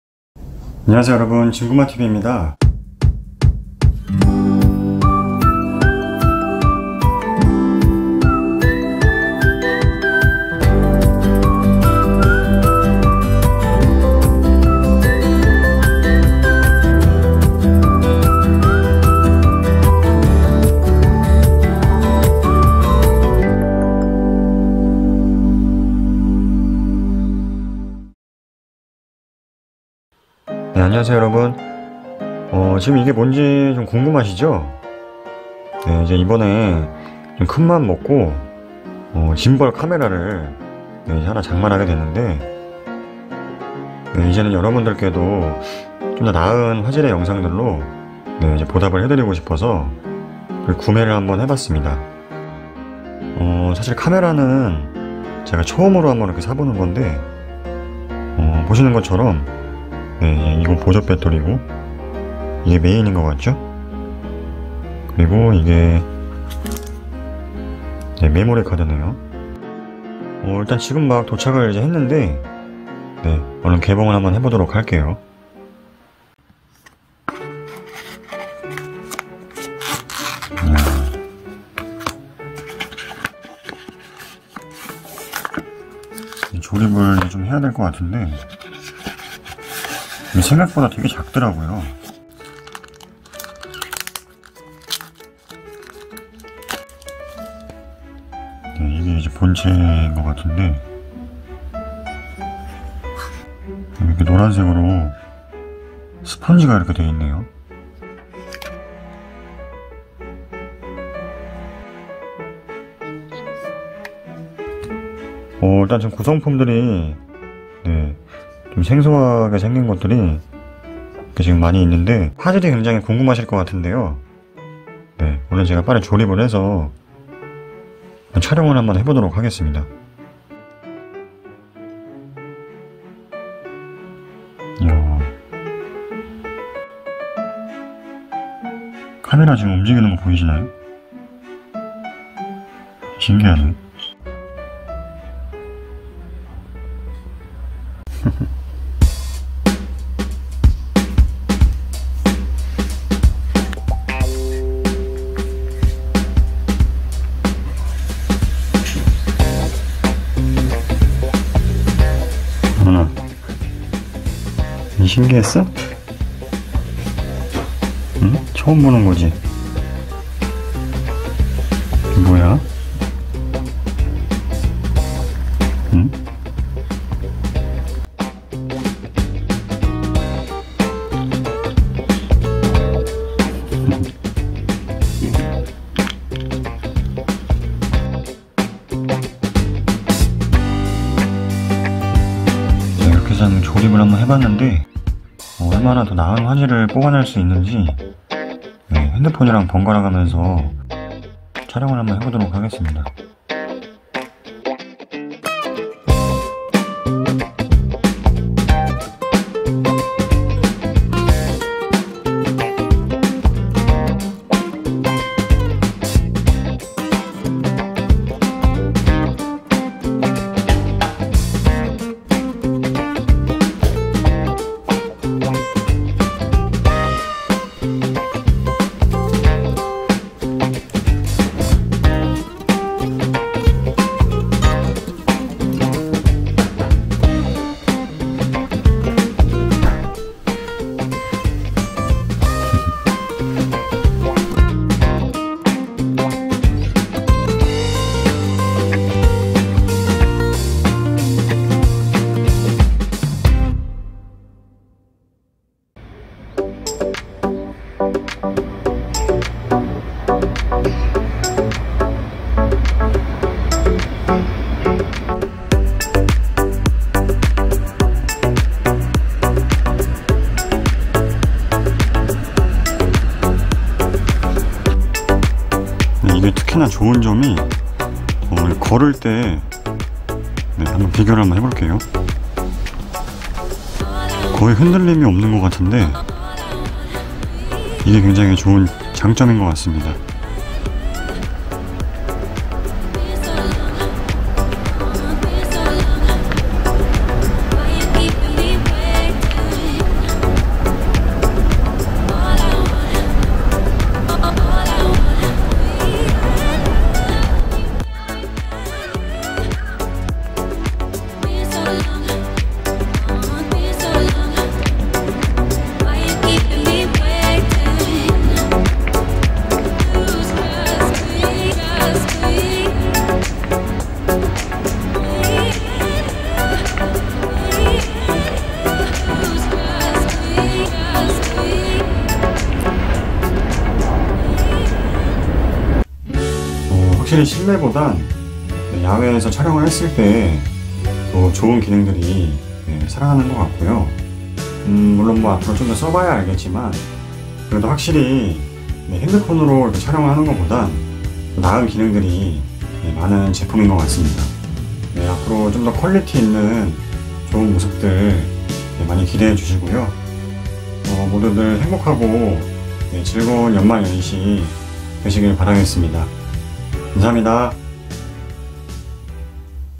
왕관앵무 마저도 반해버린 DJI 오즈모 포켓2 카메라 구입 언박싱 영상 feat.내돈내산 [진구마TV]
여러분들의 많은 성원과 응원에 힘입어 저도 보답을 해 드리고자

진구마TV 최초로 DJI 오즈모 포켓2 라는 짐벌 카메라를 구입하게 되었습니다.

제가 직접 구입 (내돈내산)을 해서 제작한 언박싱 영상이기에

절대로 오해는 없으셨으면 좋겠습니다~

모두들 행복하고 즐거운 연말연시 되세요~💕

왕관앵무 마저도 반해버린 오즈모 포켓2의 첫 사용 후기를 남겨 봅니다.